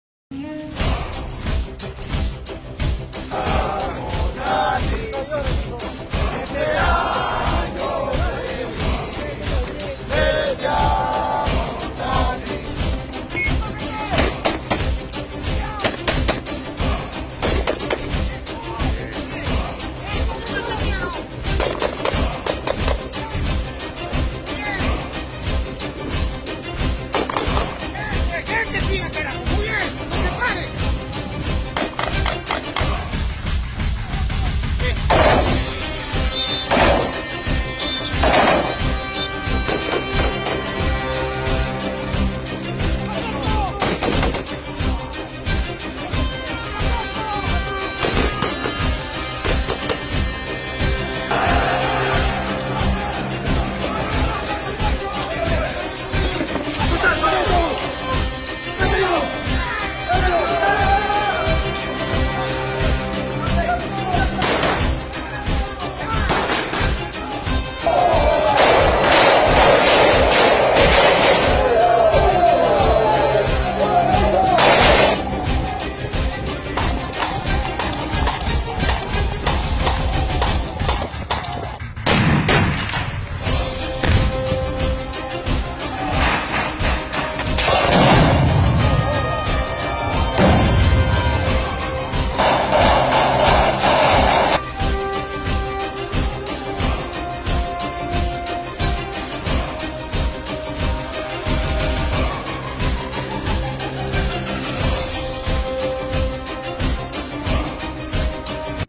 Entrenamiento de los Infantes de Marina.
Cómo es un Infante de Marina.